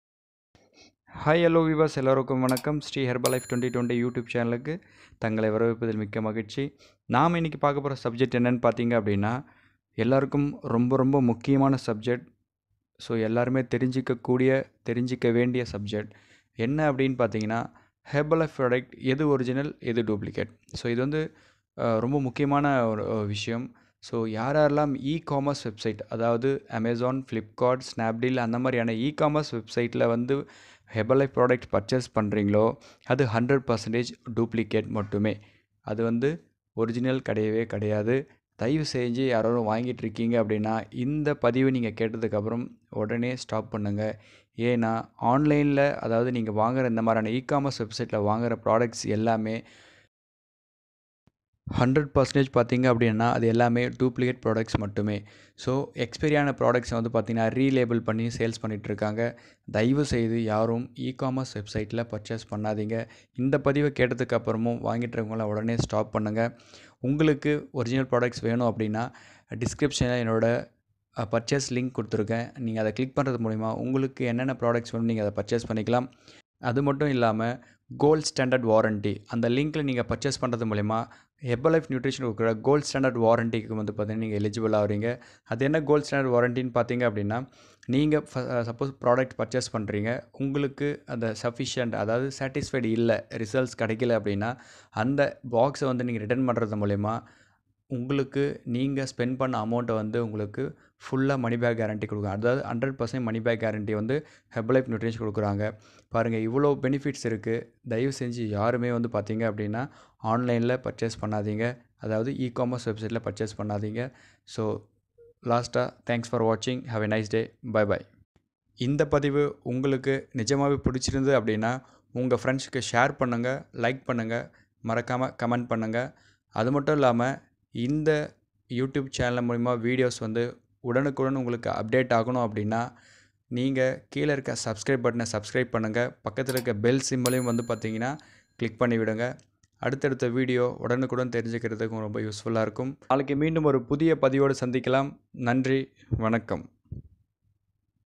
SriHerbalife2020-call@7094408287. எப்படி கண்டுபிடிப்பது? How to Identify Original or Duplicate.
Herbalife Nutrition

Herbalife Products available in E-commerce website, but that's fully Fake products.